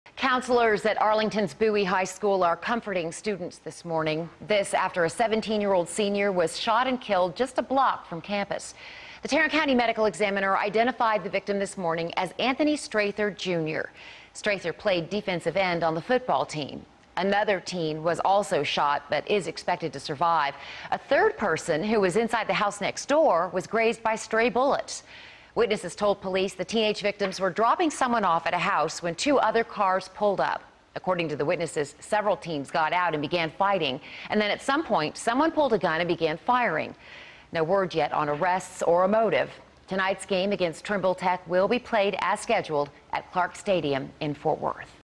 Police Identify Bowie High School Football Player Shot, Killed In Arlington
Seventeen-year-old Anthony Lemelle Jr. Strather was shot and killed just a block from Bowie High School.